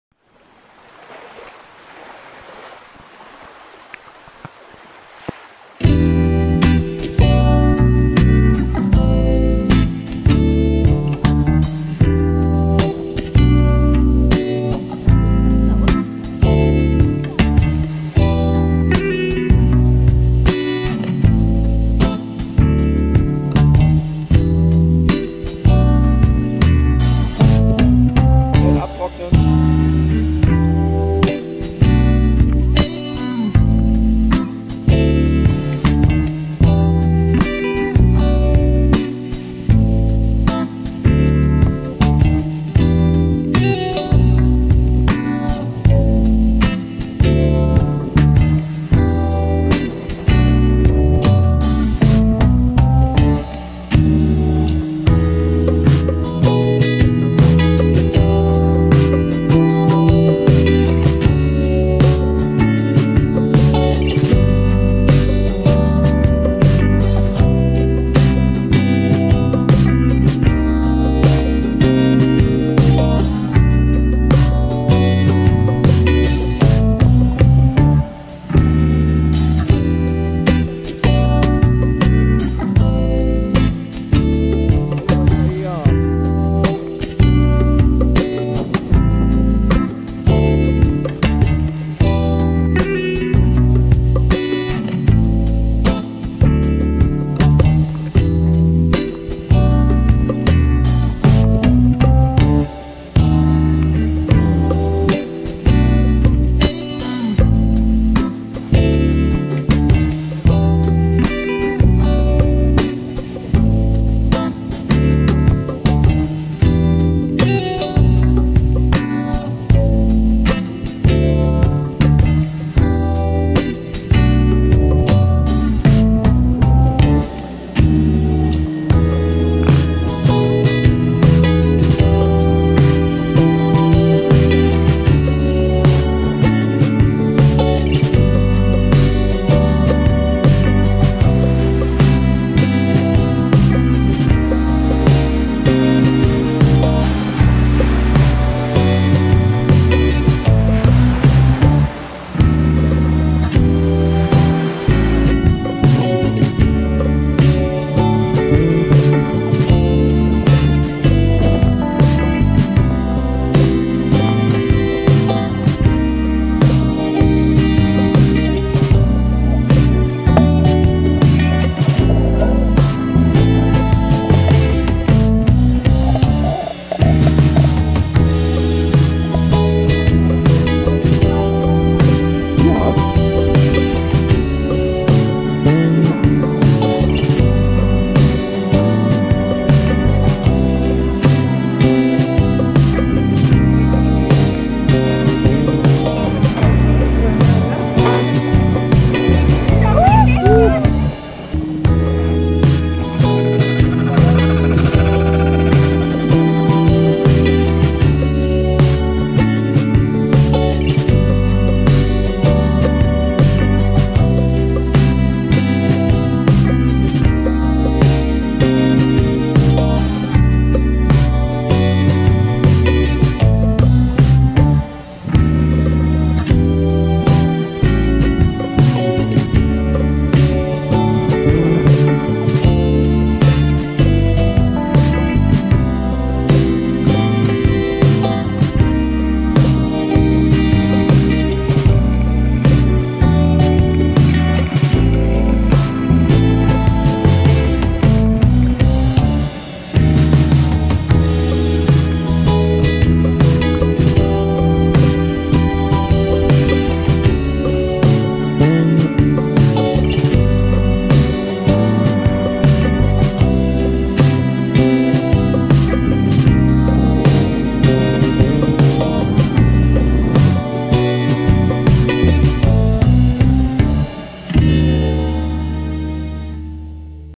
Familienurlaub am Limfjord in Dänemark
Der blaue Limfjord ist beeindruckendes und vielseitiges Urlaubsparadies! Eiszeitliche Hügel und lange Sandstrände, schöne Radwege, die Mønsted Kalkgruben oder die Insel Fur mit dem Urzeit-Museum warten auf Ihren Besuch.